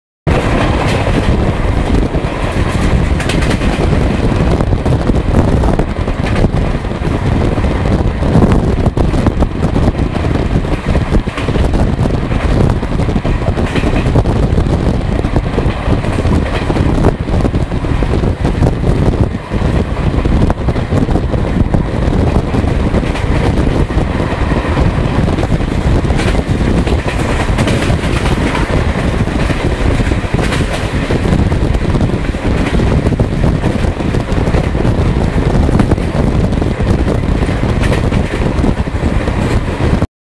SRC WAP4 ripping apart Nekurseni
SRC Red Monster blasting away with Duronto ripping past Nekurseni
Train: 12245 Howrah Yesvantpur Duronto Express
Loco Incharge: 22254 SRC WAP4
Date: 01st Dec, 2012
Time: 13:09 hrs
Location: Nekurseni, SER, West Bengal
Camera: Nokia C7 HD mobile Camera